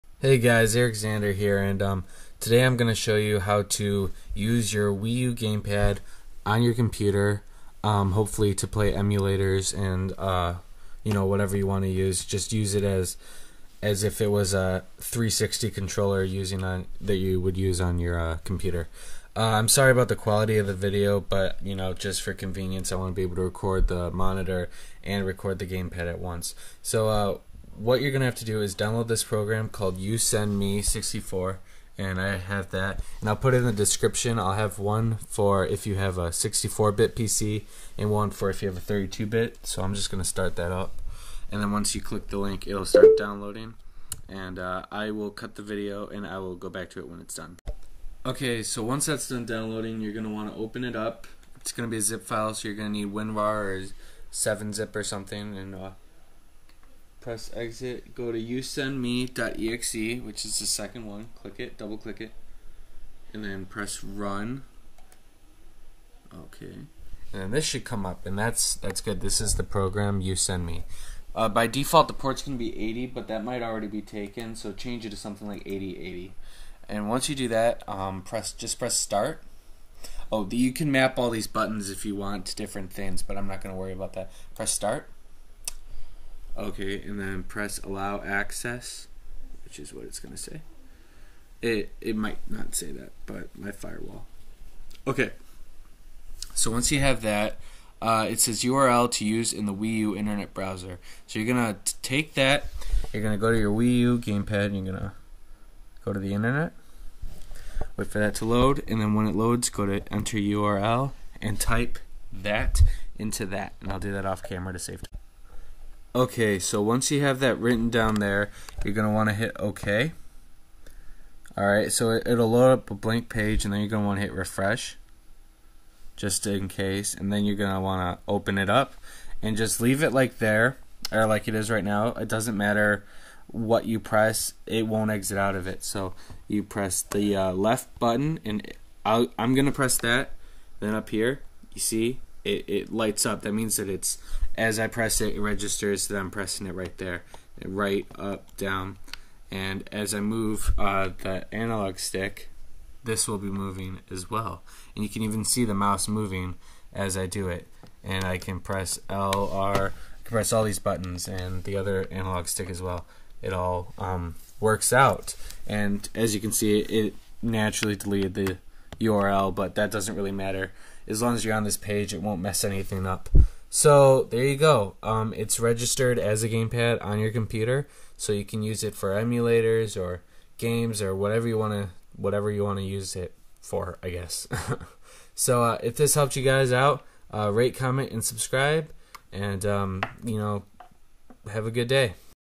How to use a WiiU Gamepad on your PC
Hey guys, today I'll show you how to connect your Wii U Gamepad to your PC to use it as a game controller to play emulators, PC games, or really do whatever you want with it!

And guys be sure to give a huuuuuge thanks to fellow YouTuber MrCrayon2000 for creating this application.  Without him, this video wouldn't be possible.

Links!

If you don't know if you have 64x or 32x, click this

If you are unable to open .rar or .zip files download Win-rar